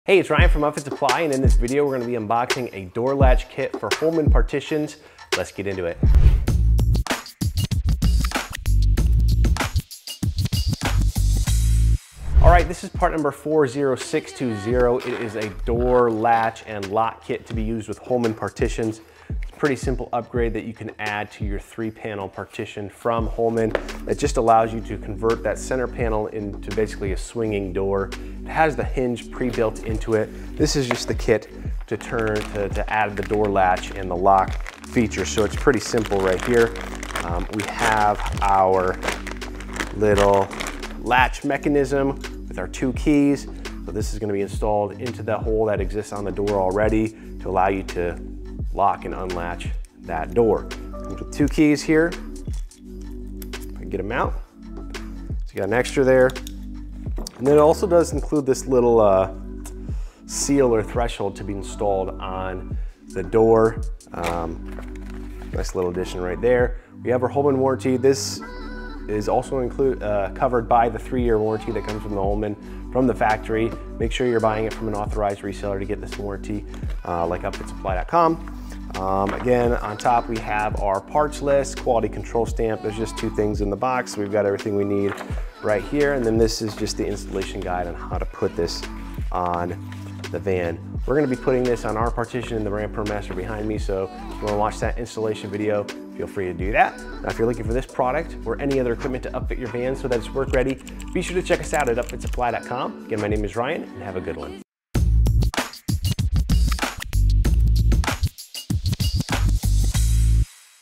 Unboxing Holman Partition Door Latch Kit
In this video, we unbox a Holman door lock/latch kit to convert the center panel on their partitions to a swing door.

Holman Part: 40620
Upfit Supply SKU: KM1-40620